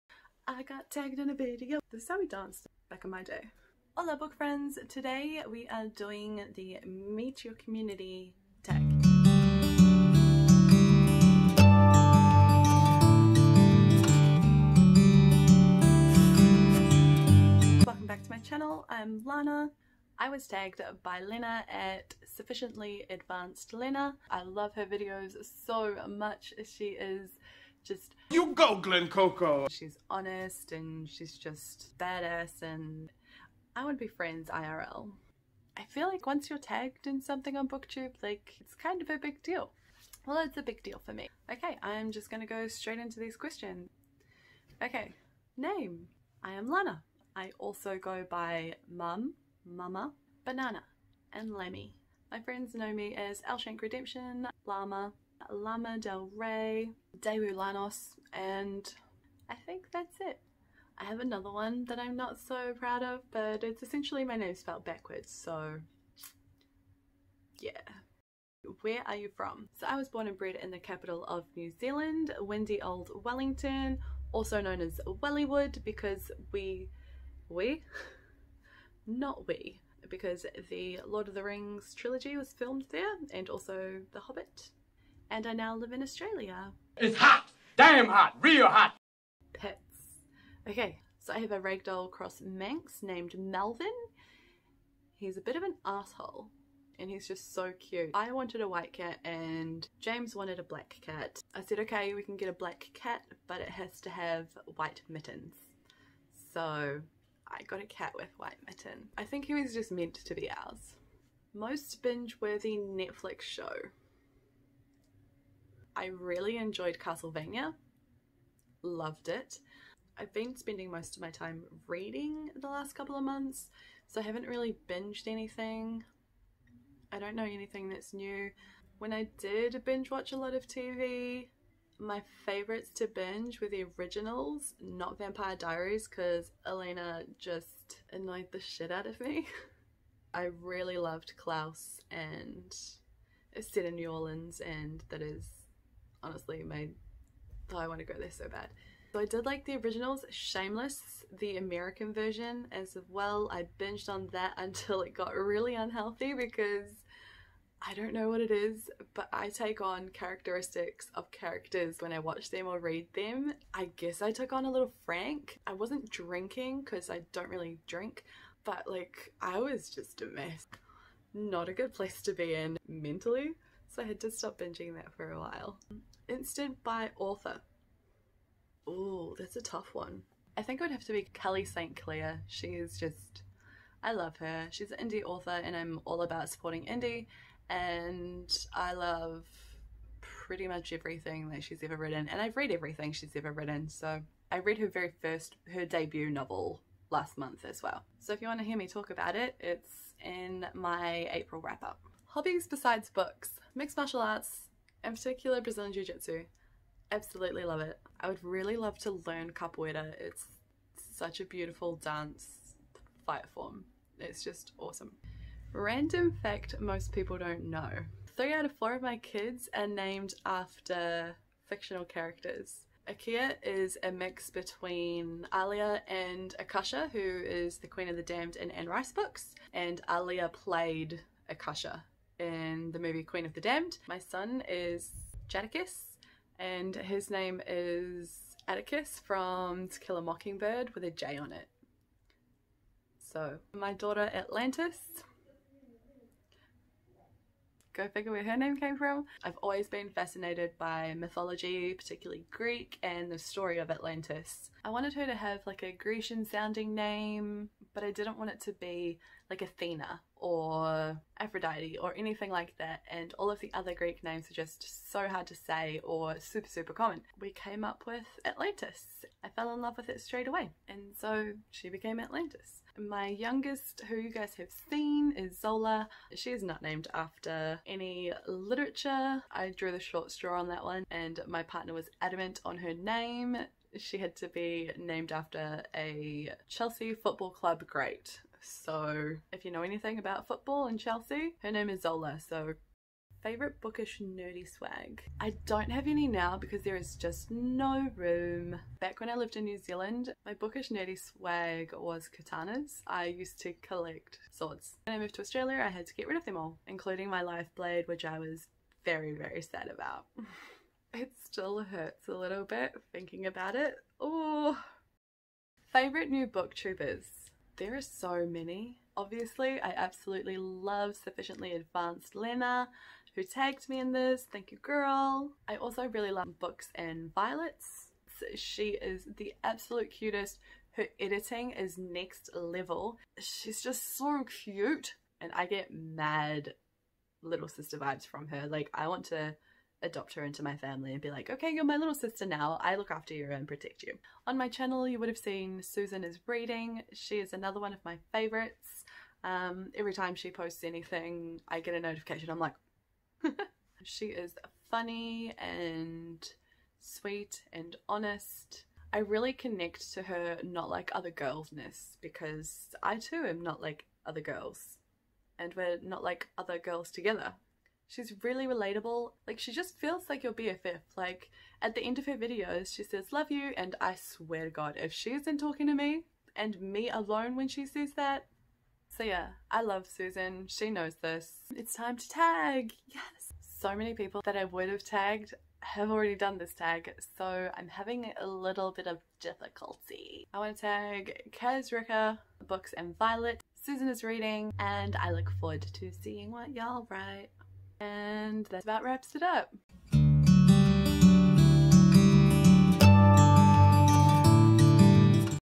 Booktube Meet The Community Tag 💖
The Booktube - Meet the Community Tag :) A tag where (you guessed it) Meet New Booktubers in the Community 💛

QUESTIONS:
Name?
Where are you from?
Pets?
Most binge-worthy Netflix show
Instant-buy author?
Hobbies besides books?
Random fact that most people don’t know?
Favourite bookish/nerdy swag
One book/series you love but no one talks about
Favourite new Booktubers? (1 - 3)
& Tag a few people!